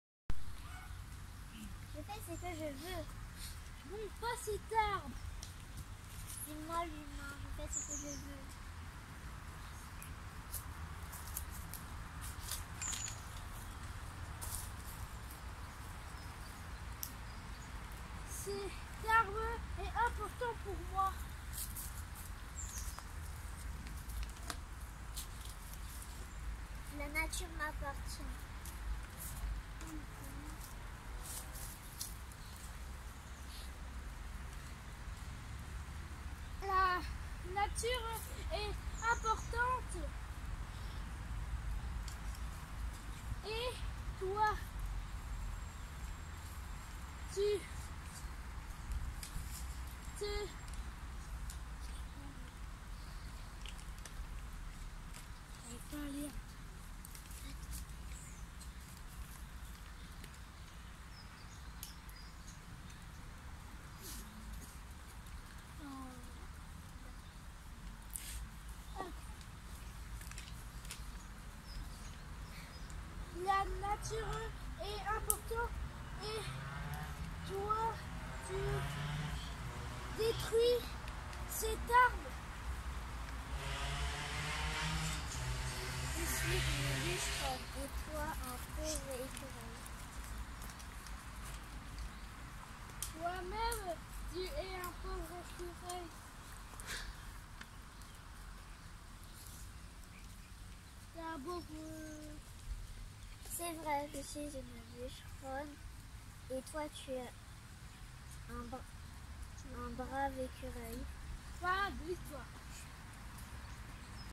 école du dehors, le nid de l'écureuil 2, Saint Joseph, Lanester
découvrez la suite de l'histoire narrée devant le groupe classe:
" Ce matin, Bruno le bûcheron rejoint la forêt muni de ses outils. Il a pour mission d'abattre l'arbre sur lequel Eric l'écureuil a construit son nid. Bruno s'approche de l'arbre. Tout à coup, il entend Eric l'écureuil hurler:

"Noooooonnnnnn! Pas cet arbre!"